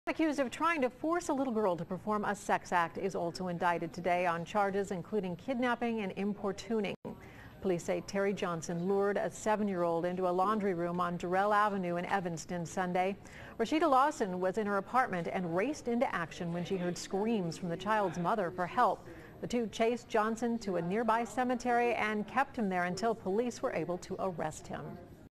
Man accused of attempting sex act with child%2
EVANSTON, Ohio (WKRC) -- A man accused of trying to force a little girl to perform a sex act was indicted Oct. 3, on charges including kidnapping and importuning.

Police said Terry Johnson lured a 7-year-old into a laundry room on Durrell Avenue in Evanston Sunday. Rasheeda Lawson was in her apartment and raced into action when she heard screams from the child's mother for help.

The two chased Johnson to a nearby cemetery, and kept him there until police arrested him.